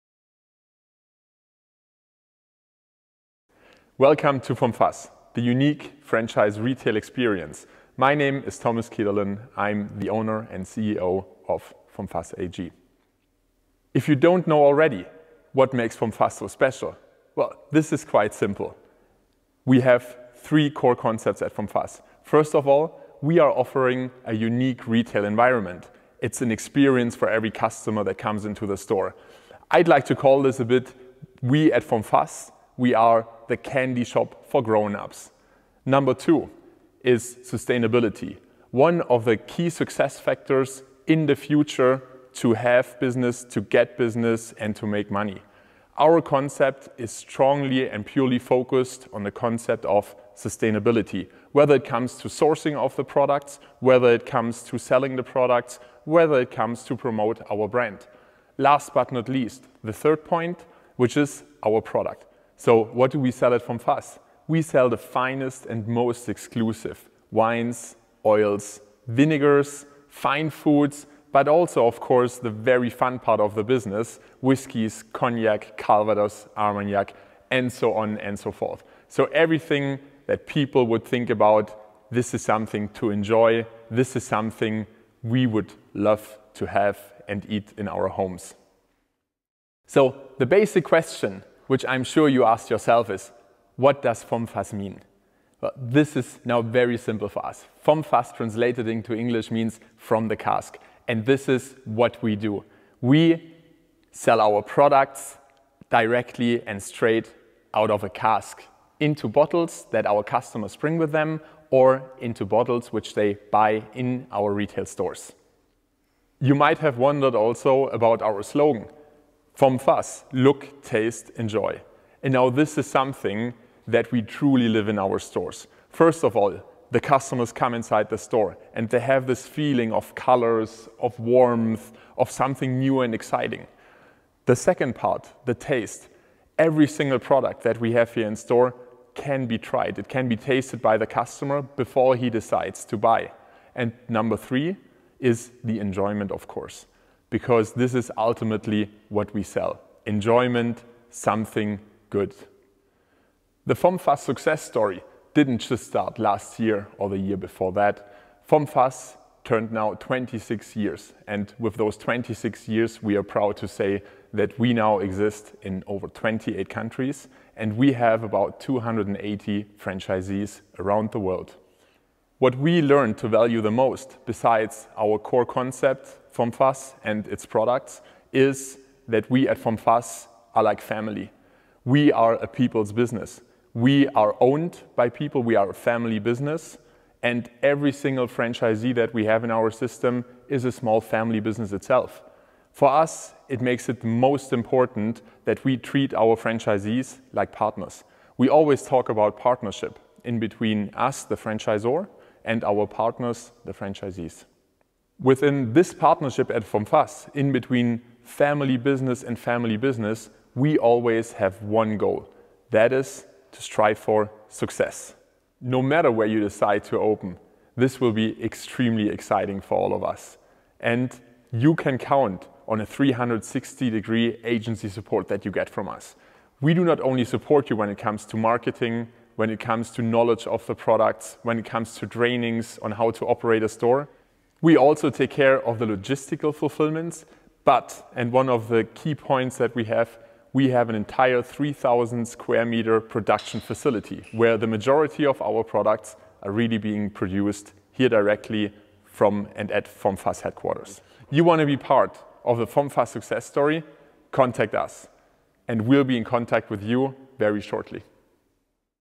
Welcome to vomFASS | Franchise Video
In this video, you can learn about the vomFASS franchise opportunity and see the steps it takes to become a vomFASS franchise partner.